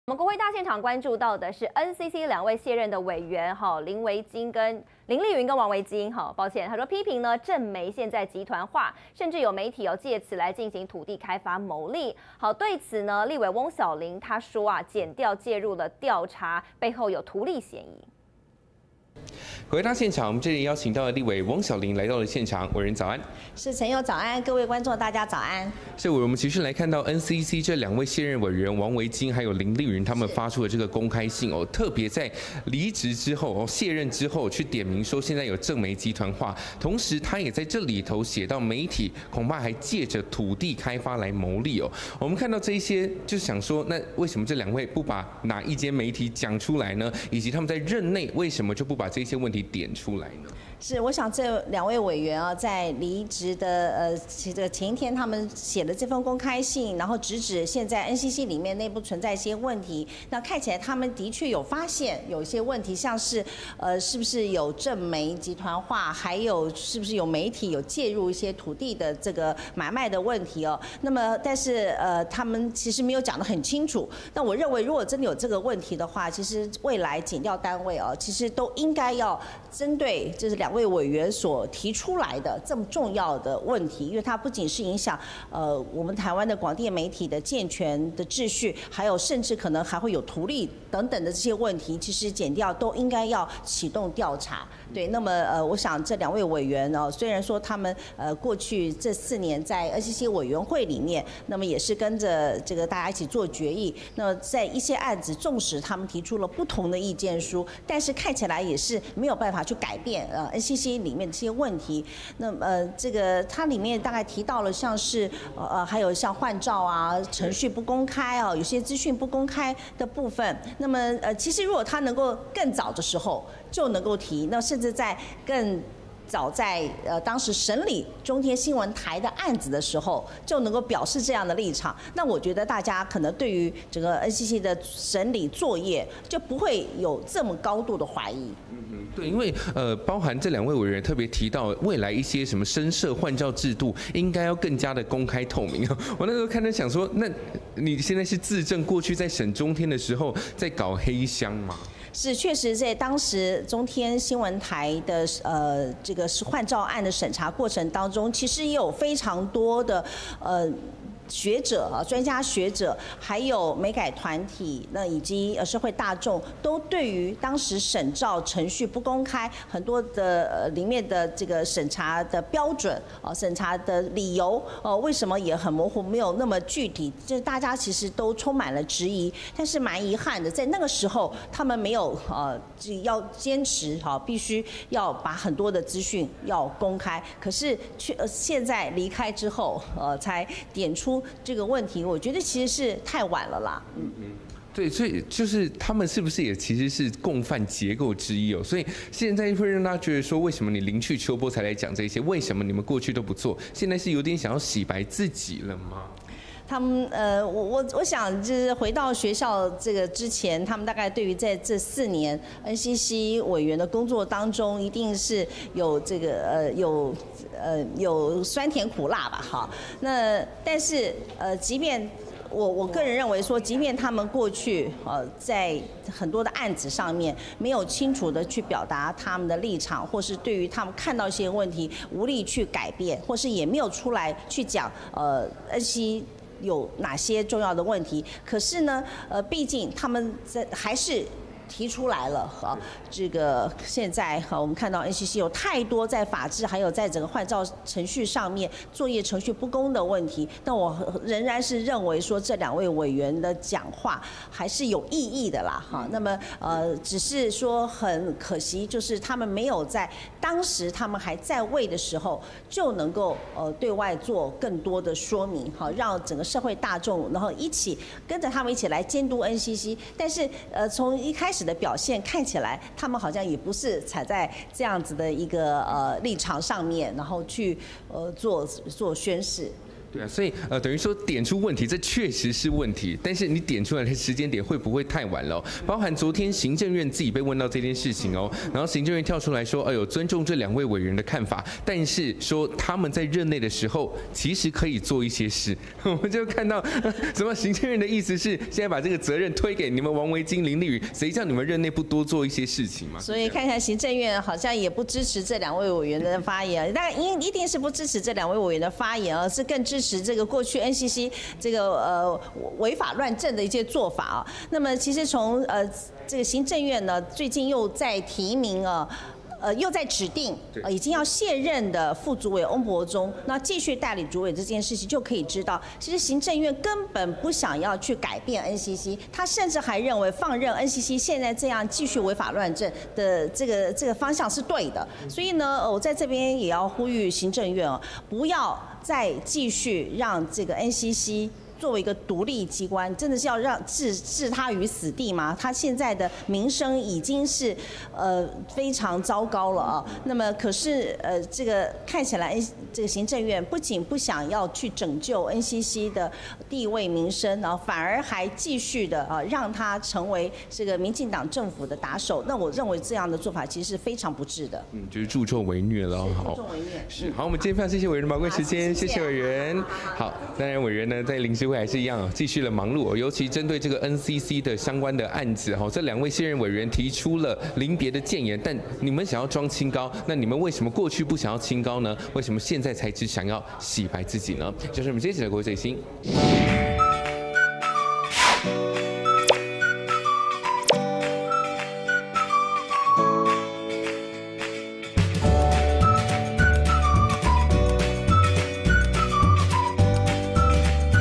【每日必看】王維菁.林麗雲卸任裝清高? 翁曉玲:離開才點出問題!｜換照機制籲"公開透明" 翁:中天換照資訊不透明 20240802
00:00 政媒集團"藉土地開發牟利" 翁曉玲:恐涉及圖利!
02:33 換照機制籲"公開透明" 翁:中天換照資訊不透明
03:53 王維菁.林麗雲卸任裝清高? 翁曉玲:離開才點出問題!